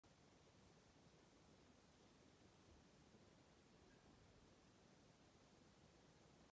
Blu-ray/Book/Figure Update for June 15, 2023
I got a Qposket and a book from the Mall￼ and I got Danganronpa the animation limited edition ￼as a late last day of school present￼